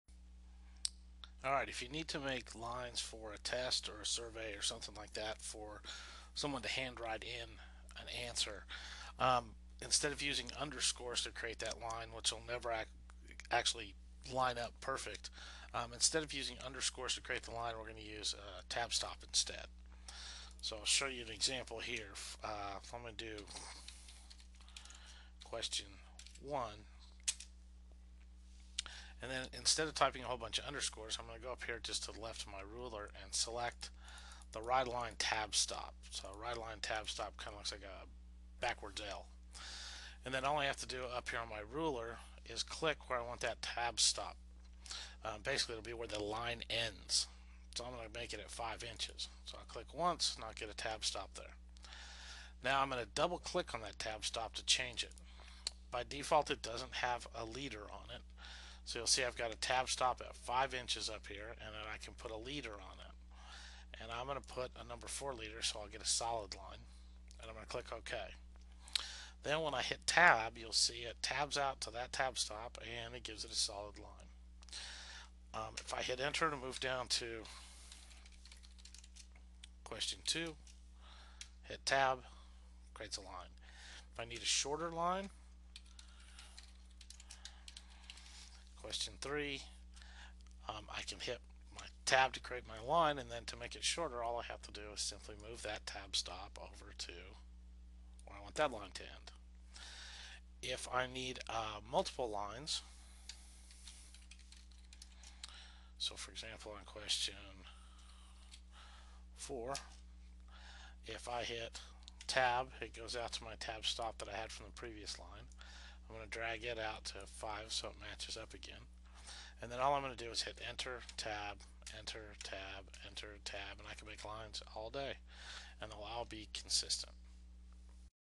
Creating lines in Word with tabs
forget underscores! Use tab leaders to create perfect lines